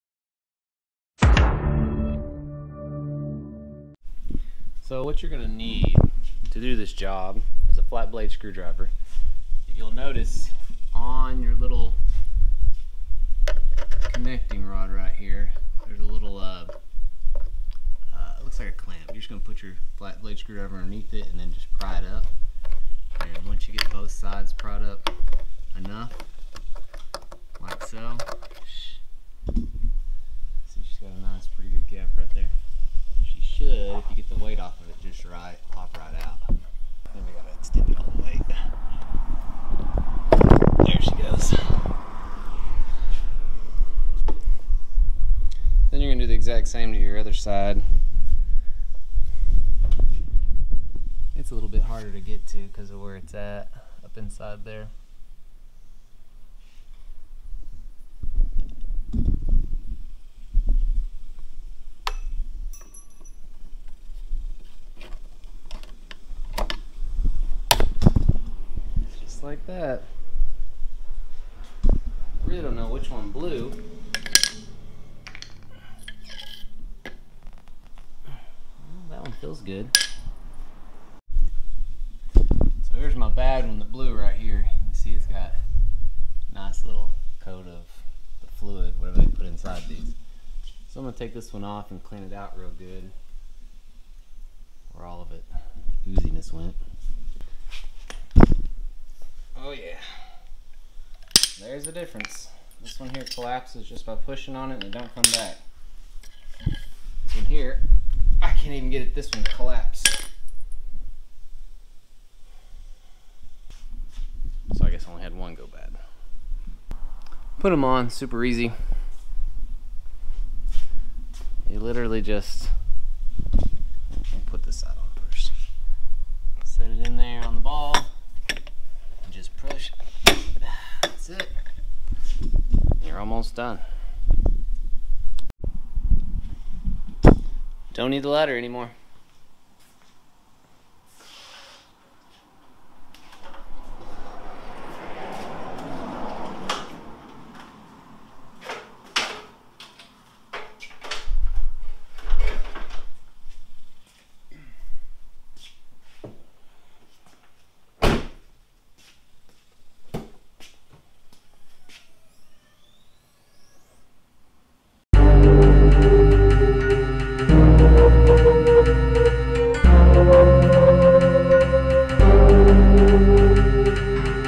jeep commander rear hatch shock replacement
replacing the rear gas shocks on my 2009 jeep commander